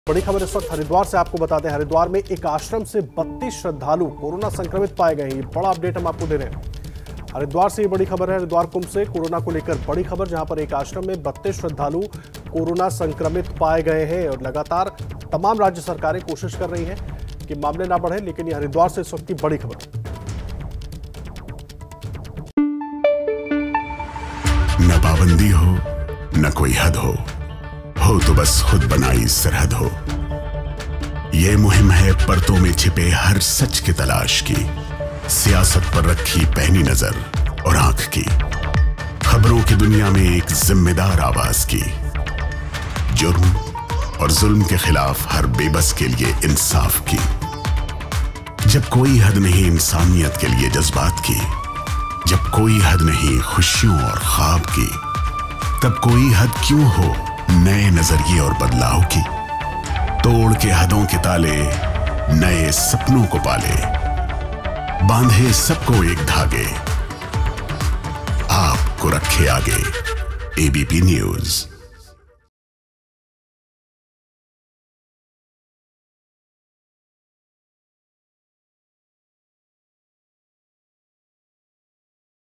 Haridwar कुंभ से कोरोना को लेकर बड़ी खबर, एक आश्रम से 32 श्रद्धालु कोरोना संक्रमित पाये गए
हरिद्वार में एक आश्रम से 32 श्रद्धालु कोरोना संक्रमित पाए गए हैं. आपको बता दें की हरिद्वार में कुंभ का आयोजन किया गया था. देशभर से श्रद्धालु यहां आस्था की डुबकी लगाने आए हुए हैं